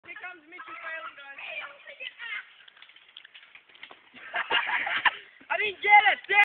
Mitchell failing 2
Mitchell attempts to jump a gap - like the last one, it dosen't end well.